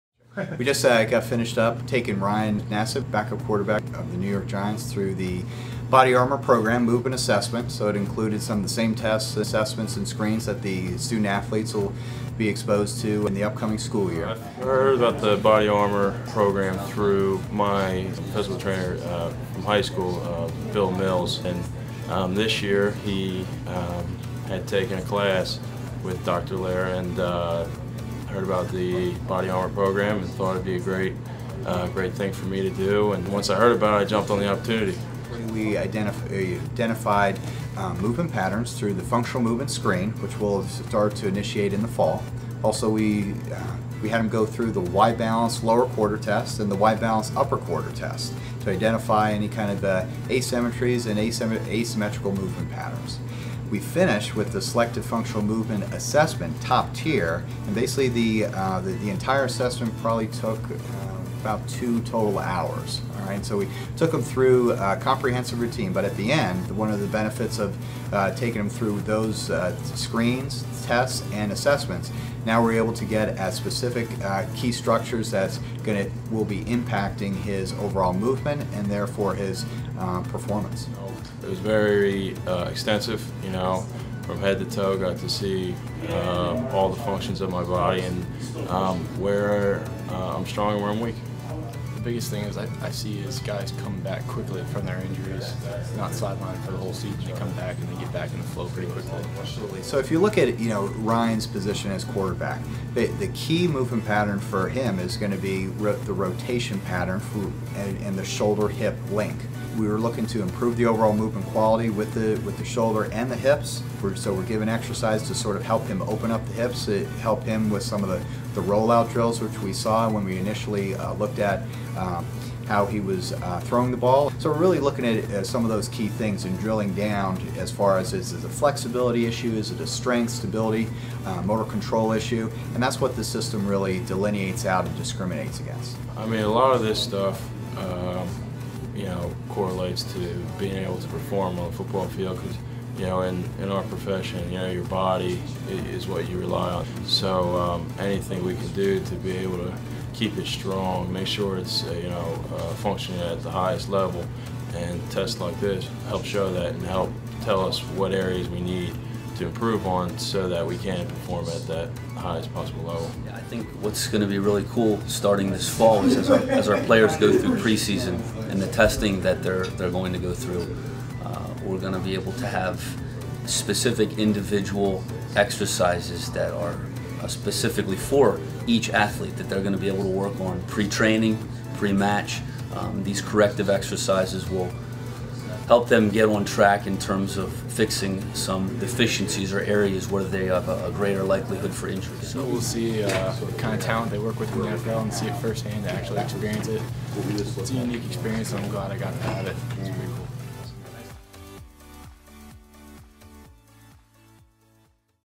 LVC Hosts Ryan Nassib for a BodyARMOR Workout
LVC welcomed New York Giants Quarterback Ryan Nassib to campus on July 7th, 2014 to participate in the Physical Therapy department's BodyARMOR program.